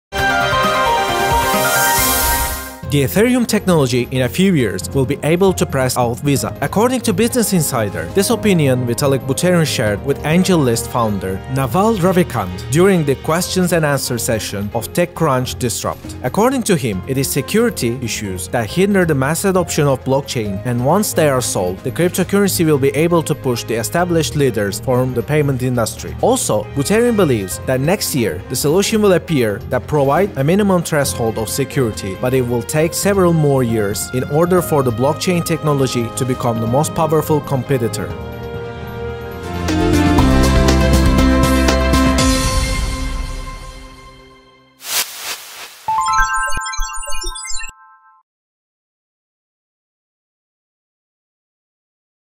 In a few years Ethereum will be able to squeeze out Visa - Vitalik Buterin
The Ethereum technology in a few years will be able to press out Visa. According to Business Insider, this opinion Vitalik Buterin shared with AngelList founder, Naval Ravinkat, during the "Questions and Answers" session of TechCrunch Disrupt. According to him, it is security issues that hinder the mass adoption of blockchain, and once they are solved - the cryptocurrency will be able to push the established leaders form the payment industry. Also, Buterin believes that next year the solutions will appear that provide a minimum threshold of security, but it will  take several more years in order for the blockchain-technology to become the most powerful competitor.